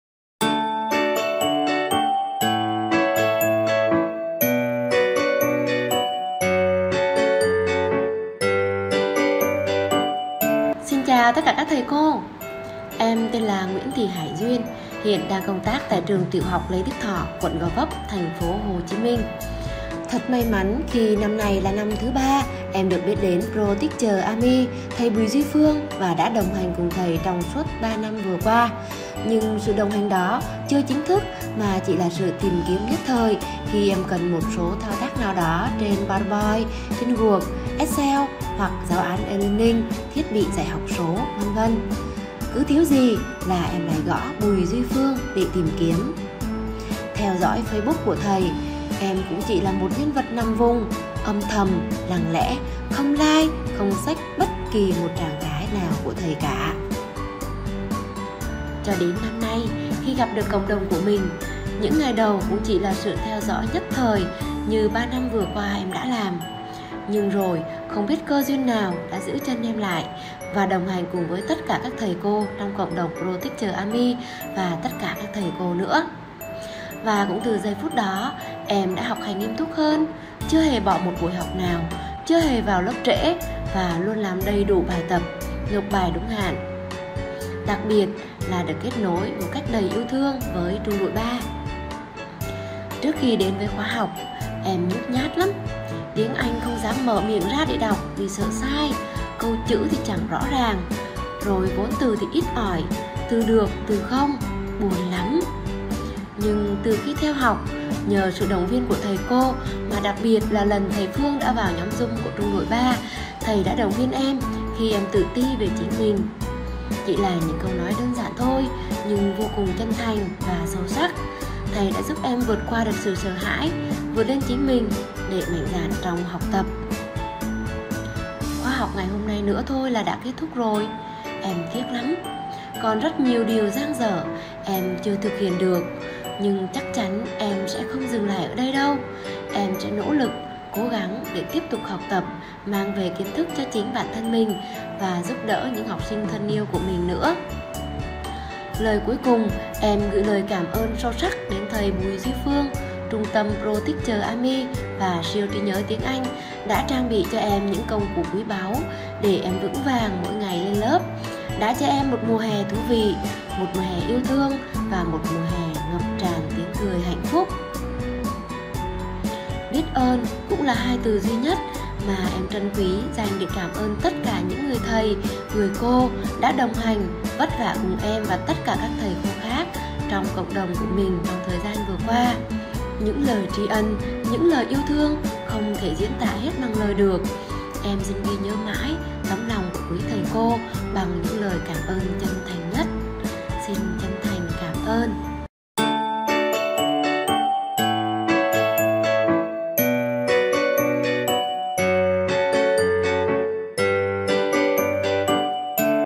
Cảm nhận cô Nguyễn Thị Hải Duyên về khóa học tin học cho giáo viên hè 2023
Người hướng dẫn:
►Teacher: Bùi Duy Phương